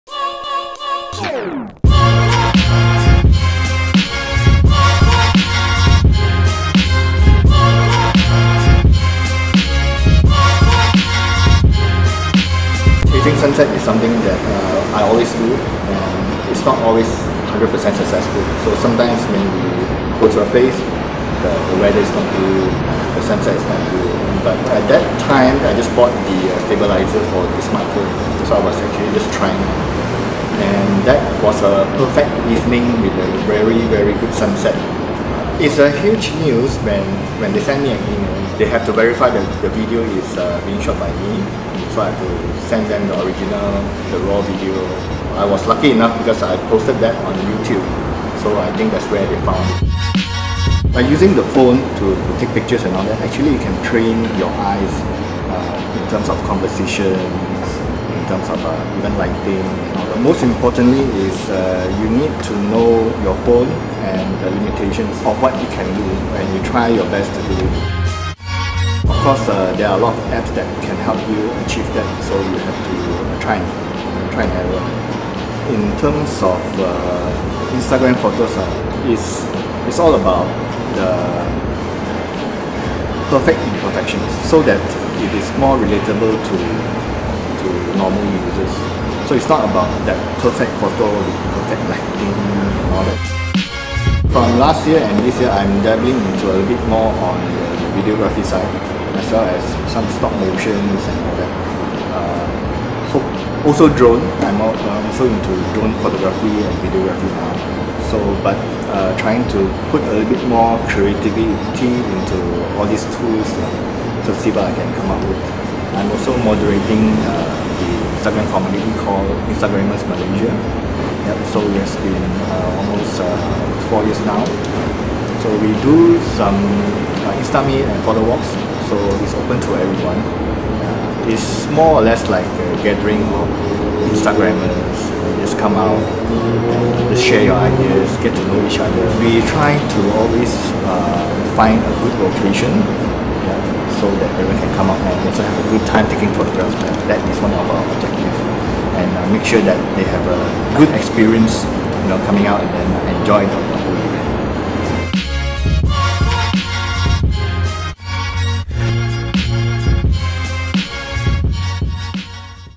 The Macallan Edition No. 2, featuring John Leong
In keeping with the spirit of creative collaboration that culminated in the conception of The Macallan Edition No. 2, August Man partners The Macallan in the following video, featuring iPhone photographer @Thrumyiphone and moderator of Instagramers Malaysia @igersmalaysia John Leong on "Fostering Collective Creativity, Passion and Craft".